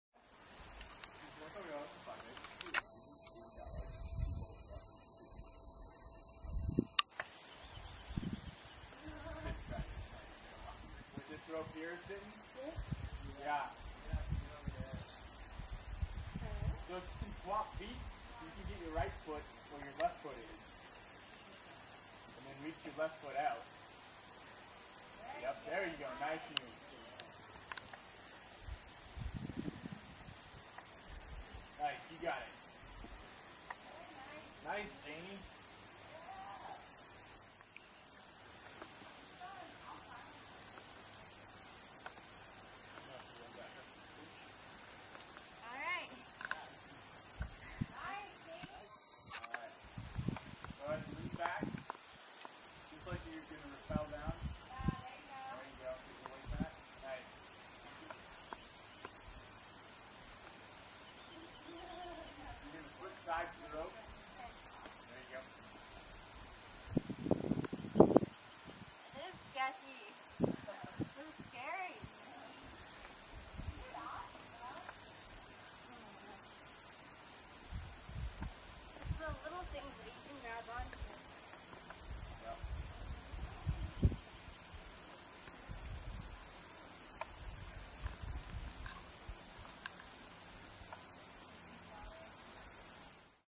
Rock Climber at Hartman Rocks
A young lady learning to rock climb at Hartman Rocks near Gunnison, Colorado.